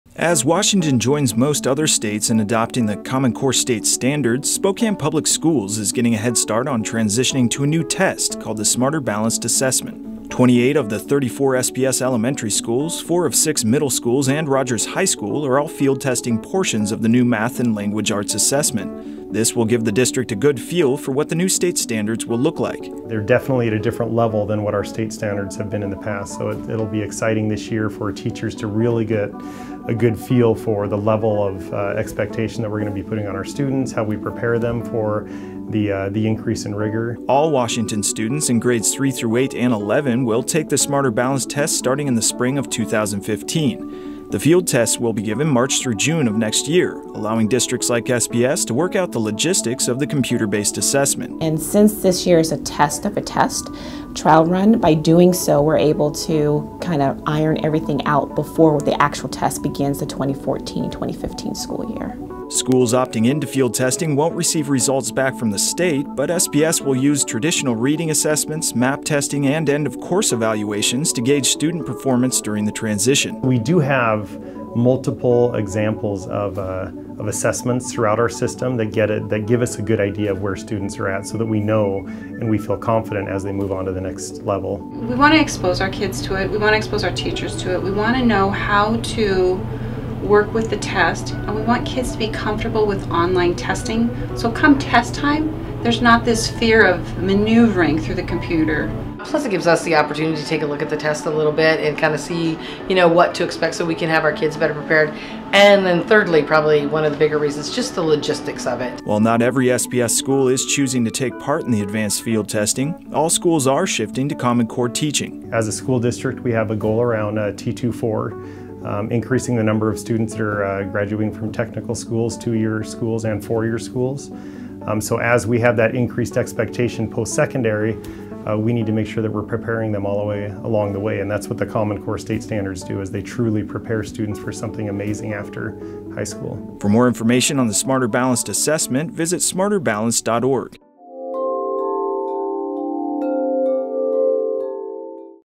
Many SPS schools to field test new Smarter Balanced Assessment
As Washington joins most other states in adopting the Common Core State Standards, Spokane Public Schools is getting a head start on transitioning to a new math and language arts test called the Smarter Balanced Assessment.